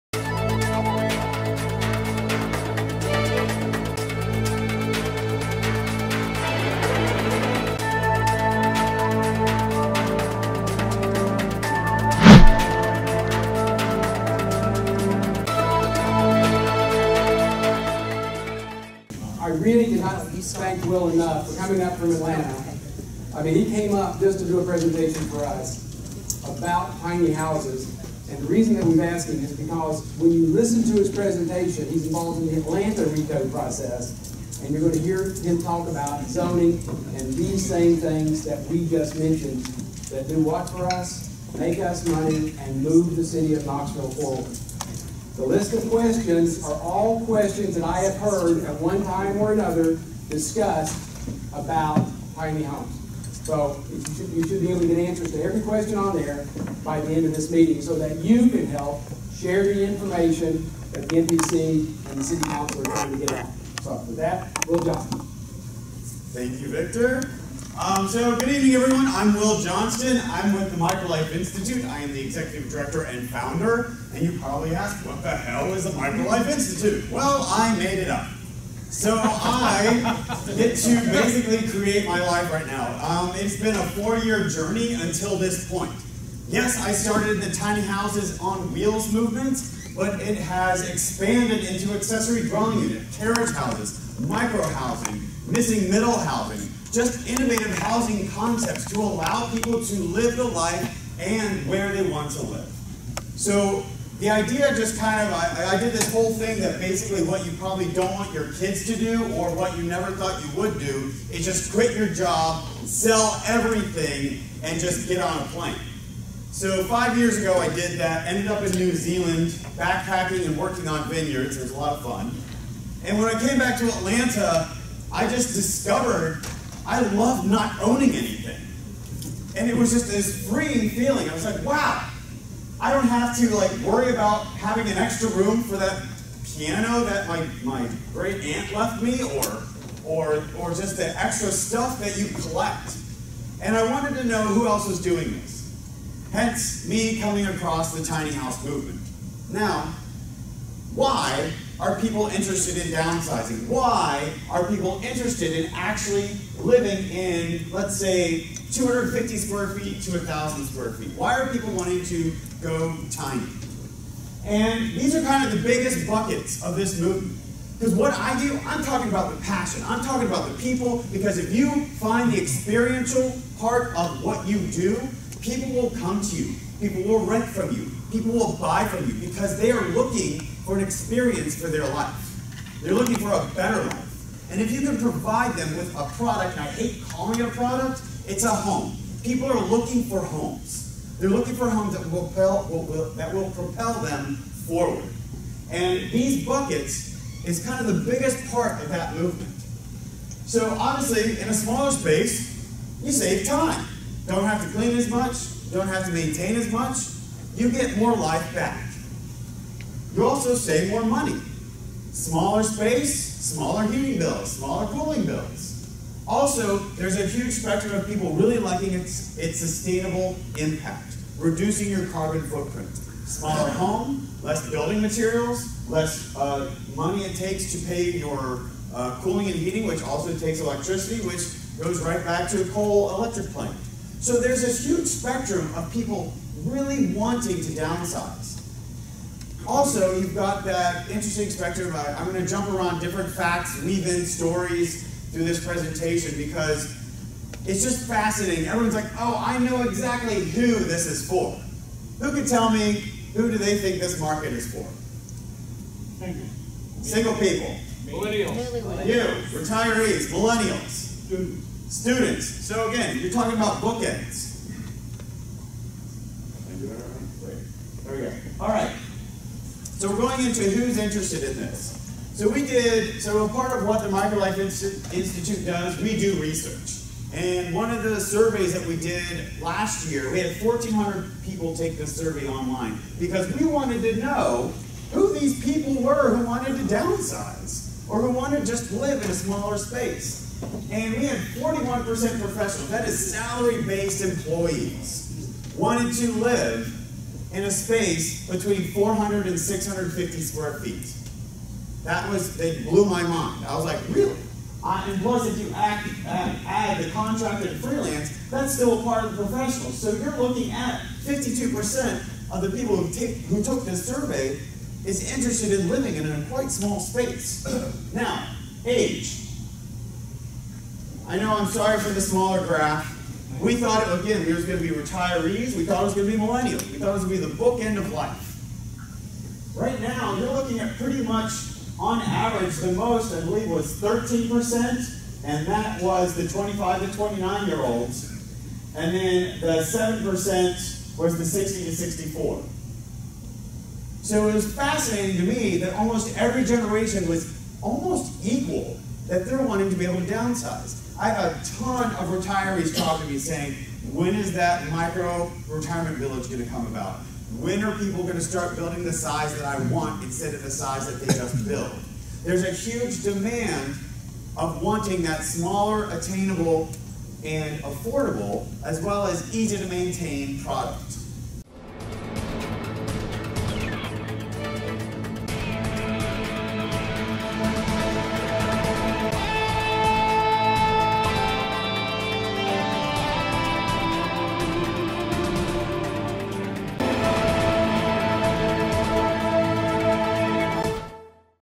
Real Estate Investing: Tiny Houses and the Micro Living Movement
At the July 10, 2018 Knox REIA monthly meeting Will Johnston, founder of the Micro Living Institute, discusses tiny houses and the micro living movement.